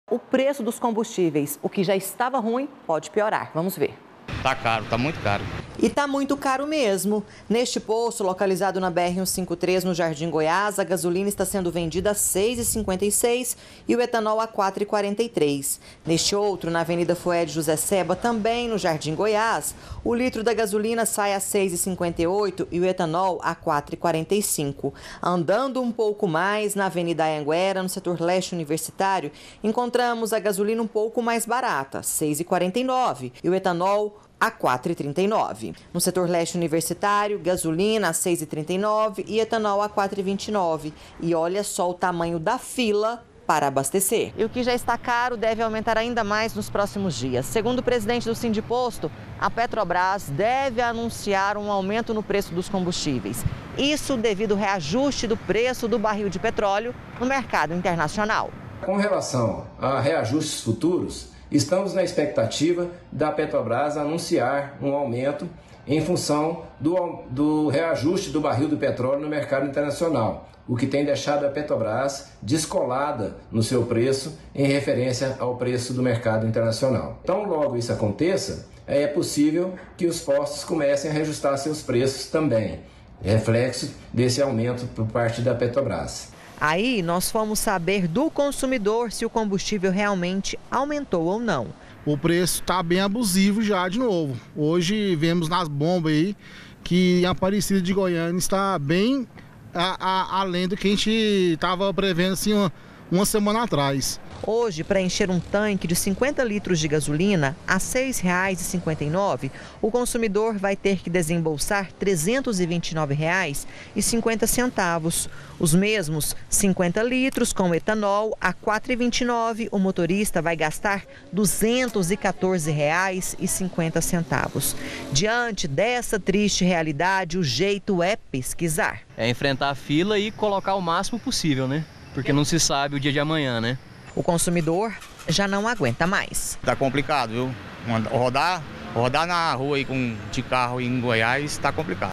JSD (07/03/22) Reajuste no preço dos combustíveis
Em alta: consumidor não suporta mais o preço alto dos combustíveis.